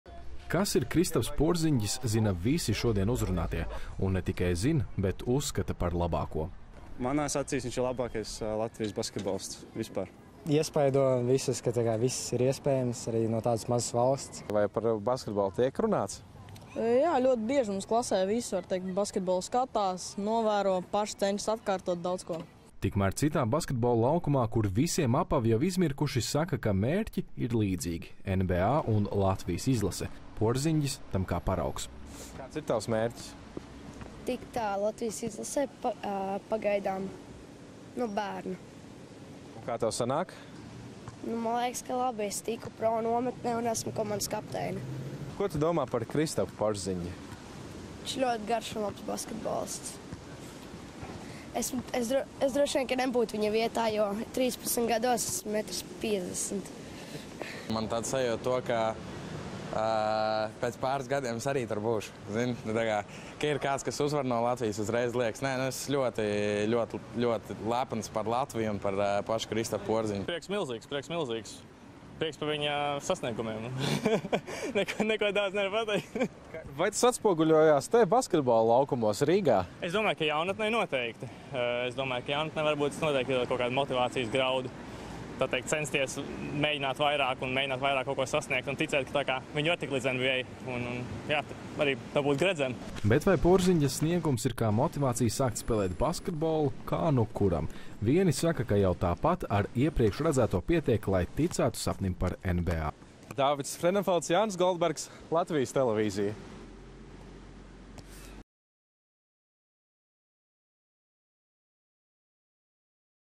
Bērni un jaunieši sajūsmā par Porziņģa panākumiem
Apsveikumu lavīna, šampanieša plūdi un nebeidzamas intervijas – tā šo rītu pavadīja Kristaps Porziņģis – Latvijas pirmais NBA čempions. Par spīti savainojumam, viņš aizvadītajā naktī devās laukumā un ar savu komandu Bostonas “Celtics” izcīnīja titulu!

Kāds gan tur brīnums, ka bērnus un jauniešus Latvijā šodien no bumbas mešanas grozā neatturēja arī lietus. Kristapa Porziņģa vārds ikvienam ir zināms – tas liek vēl vairāk lepoties ar Latviju un vēl vairāk censties basketbola laukumā.